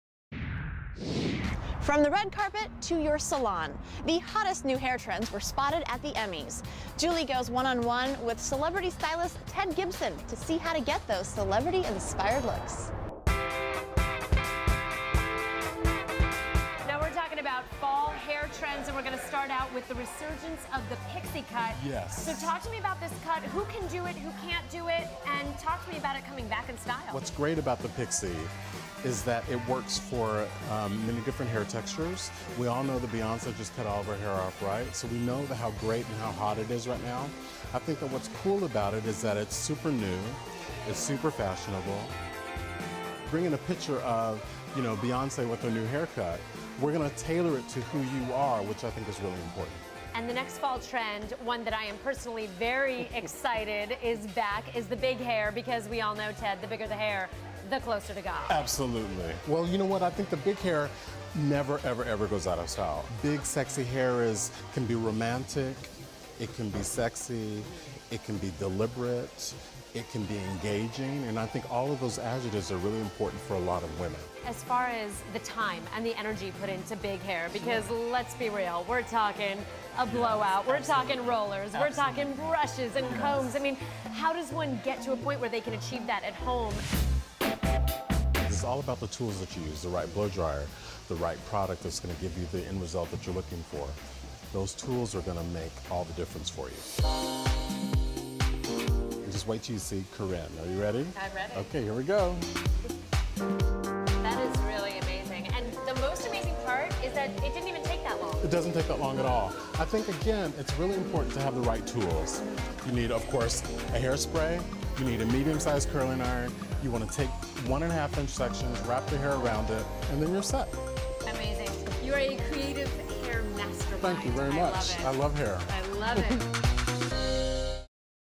Celebrity Hair Trends with Ted Gibson
Celebrity stylist Ted Gibson shows you how to get celebrity inspired looks.  OK! TV's Julie Alexandria is at the Ted Gibson Salon showing you fall hair trends like the pixie cut and big hair. He shows you the importance of using the right tools.